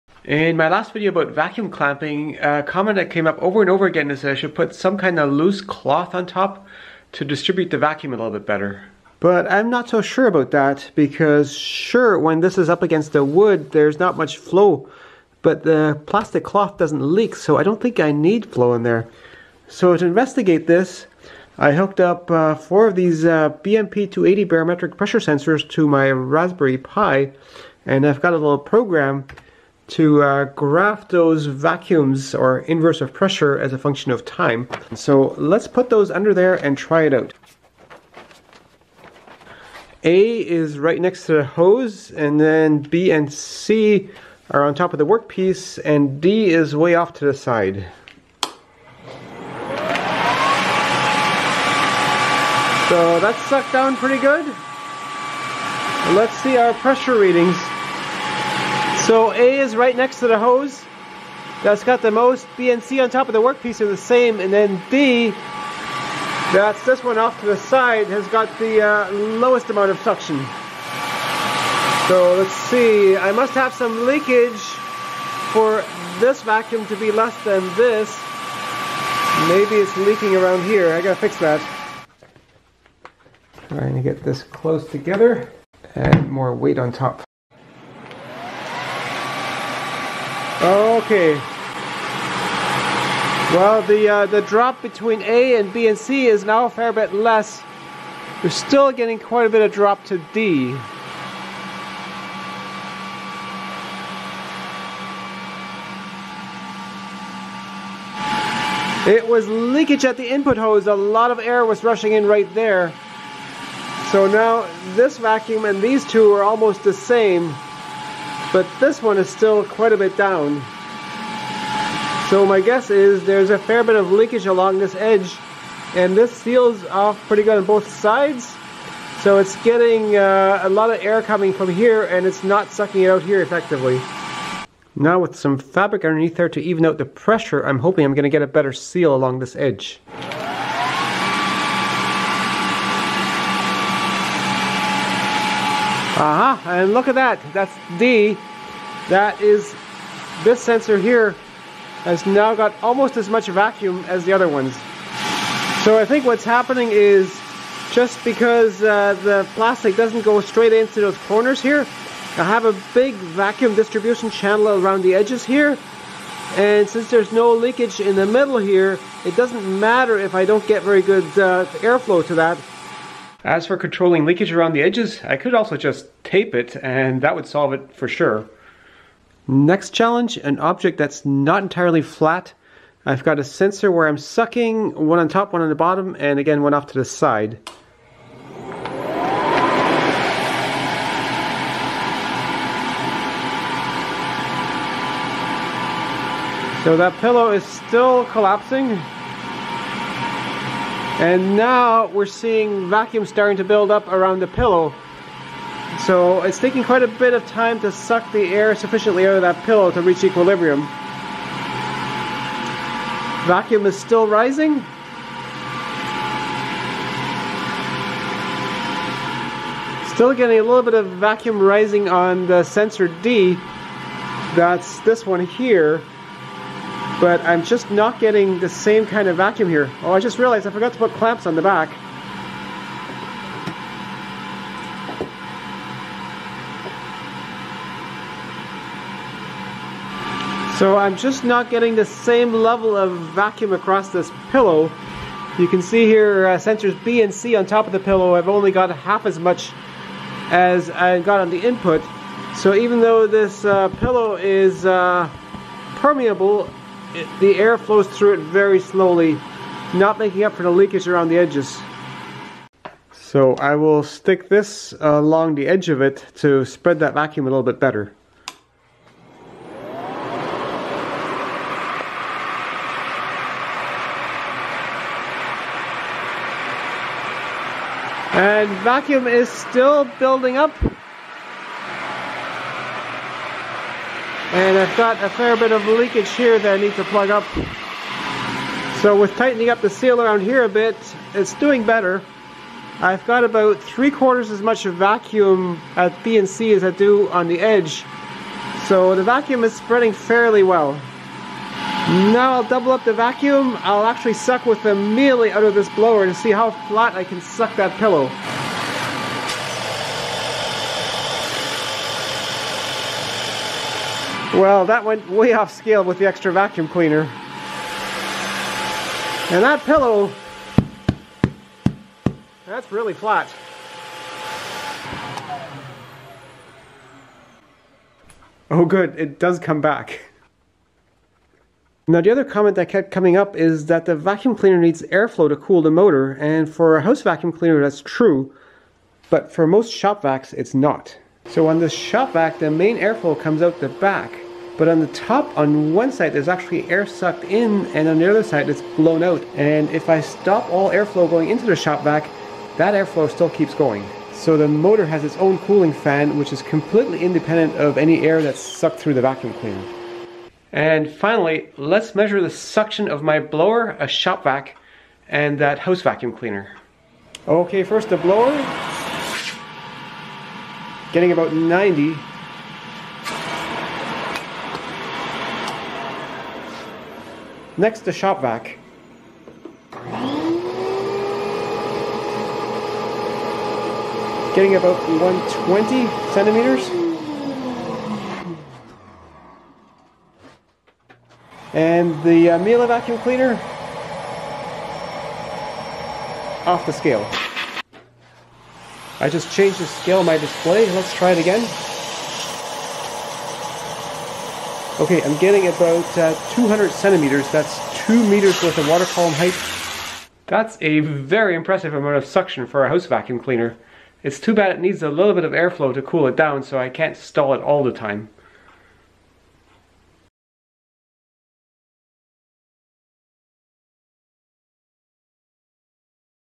Vacuum clamping suction spread: measuring vacuum distribution.
Using some barometric pressure sensors hooked up to a computer to measure how well the vacuum gets distributed over the area when vacuum clamping to determine if some "cloth" on top to spread the vacuum is really necessary.